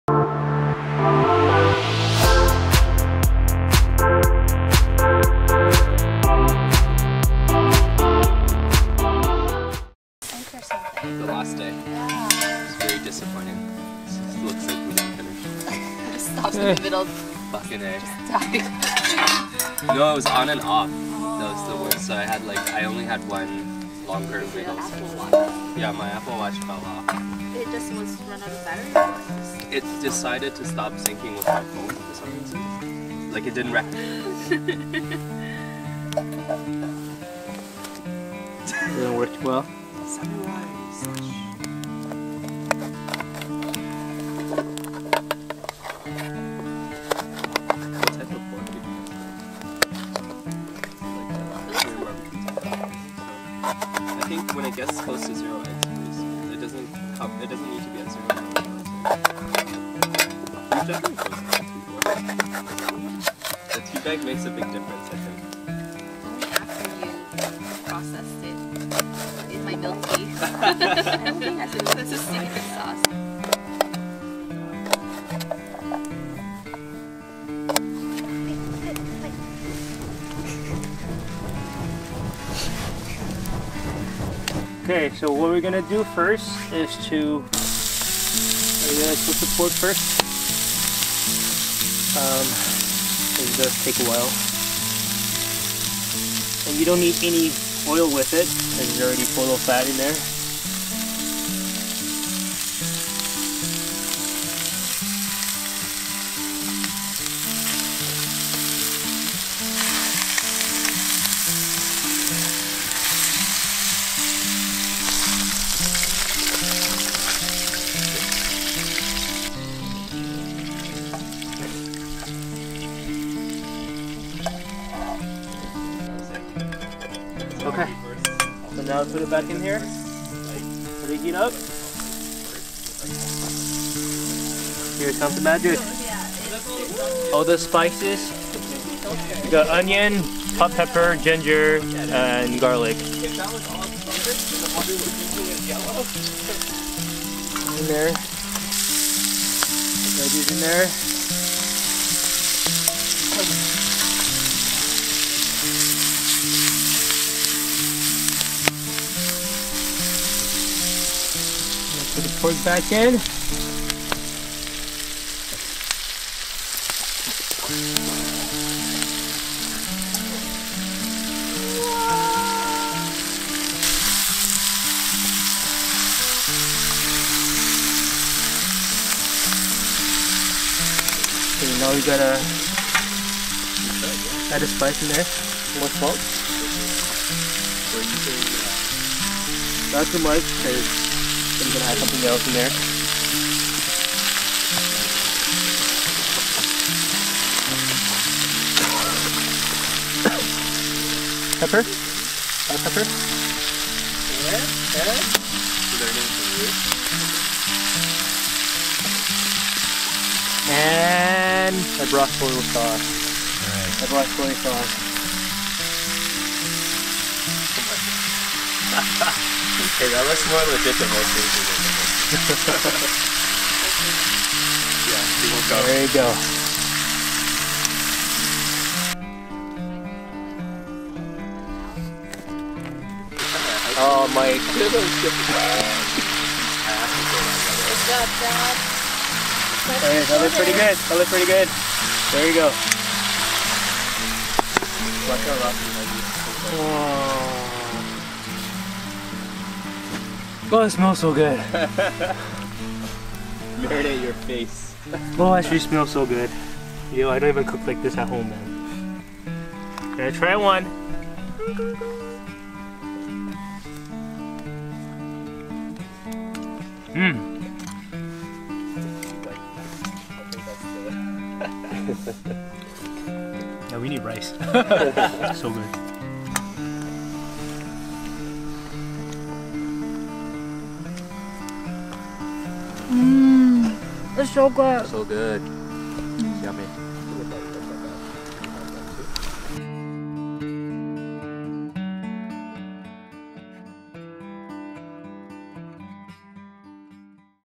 Cooking in The Wild: Stir Fried Spicy Pork Belly by The Lake
Trying different types of dishes in the wilderness. This one was done beside the Tom Thomson Lake in Algonquin Provincial Park, Ontario, Canada.